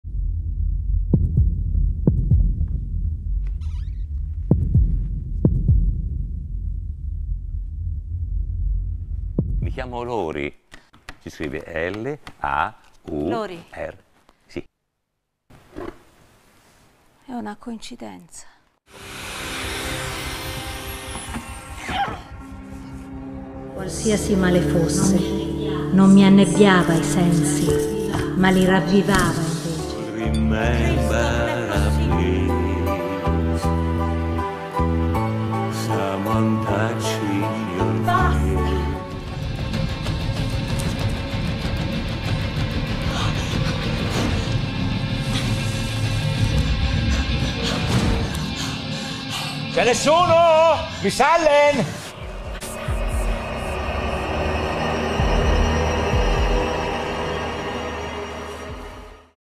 "IL CUORE RIVELATORE" di A.Neilson (2nd trailer)
Da venerdì 8 ottobre 2021, al Teatro della Visitazione, via dei Crispolti, 142, Roma e nelle settimane successive dal giovedì alla domenica (giovedì-sabato ore 21/ domenica ore 17.30) la BaBi Production di Barbara Lo Gaglio e Luca Biagini presenta:

Il cuore rivelatore

di Anthony Neilson ispirato all’omonimo racconto di Edgar Allan Poe

con

Barbara Lo Gaglio – Martina Colaprico – Luca Biagini

Scene e costumi Maurizio Balò

Regia  Federico Vigorito

Il Cuore Rivelatore di Anthony Neilson rielabora in un horror contemporaneo le pagine terrificanti del capolavoro di Poe. E’ il dramma di un’ossessione che si tramuta in follia omicida. Un giallo esemplare che non avremmo risolto senza il più involontario degli indizi: il battito di un cuore.